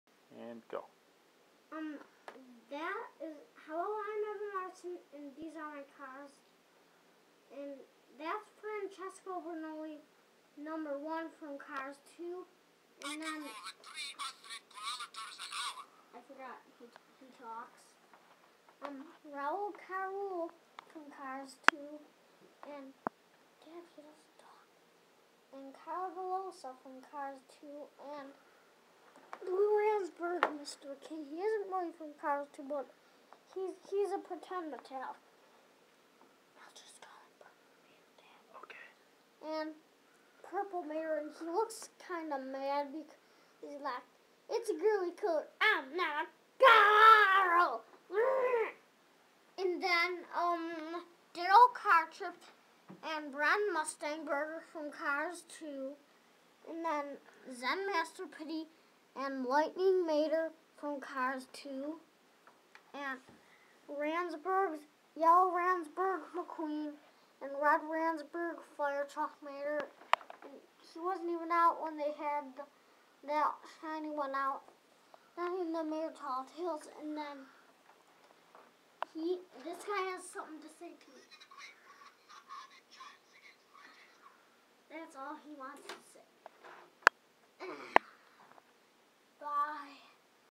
Cars 2 and a couple more
Evan talking about new cars2 cars.